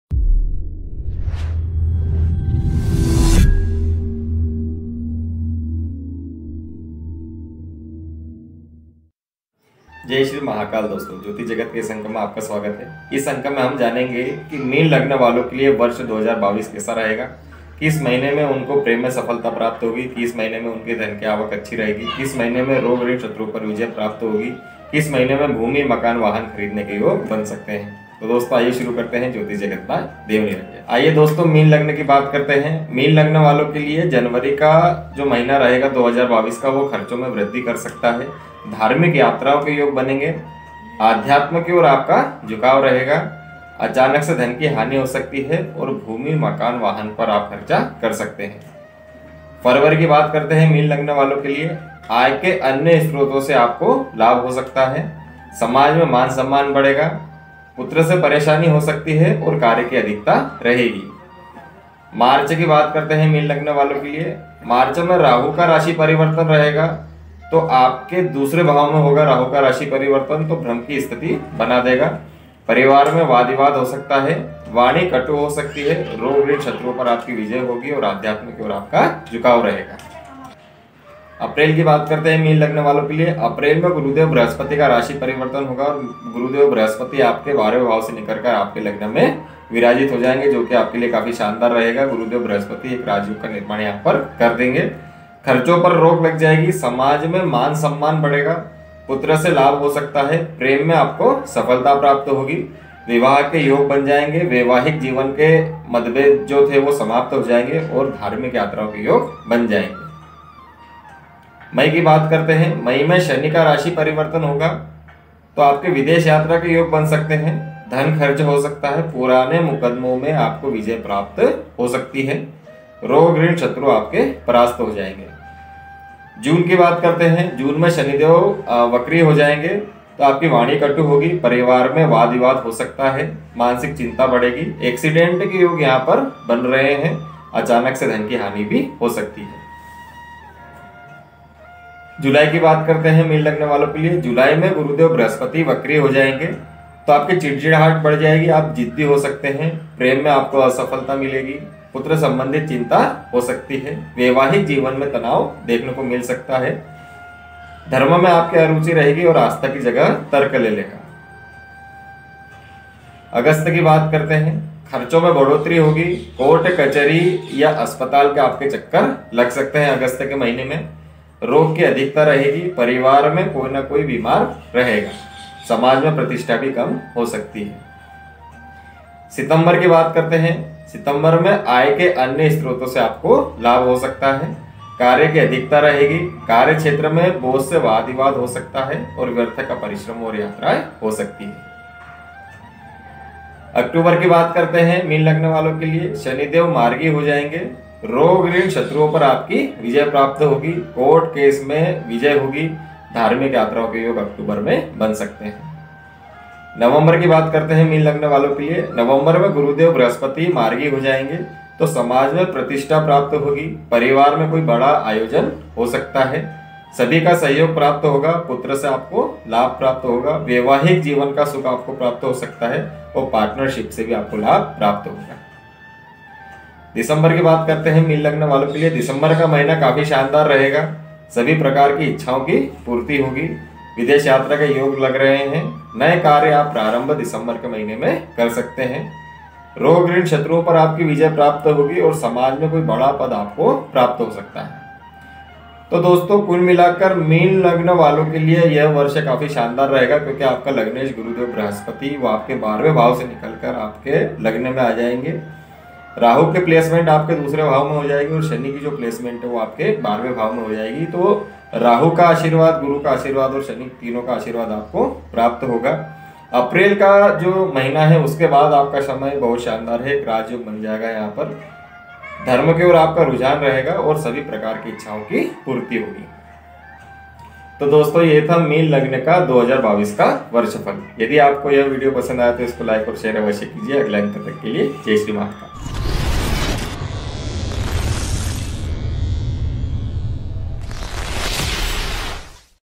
मीन सम्पूर्ण वर्षफल 2022 एवं अचूक भविष्यवाणी । Pisces horoscope 2022 by Dev niranjan
दोस्तों, इस वीडियो में जानिए की मीन लग्न वालों का वर्ष 2022 कैसा रहेगा, किस महीने इन्हें लाभ होगा, किस महीने हानि हो सकती है ।

Friends, in this video know how will the year 2022 of Pisces  ascendant, in which month they will benefit, in which month there can be loss.

- देव निरंजन
(महाकालेश्वर ज्योतिष संस्थान, देवास, म.प्र.)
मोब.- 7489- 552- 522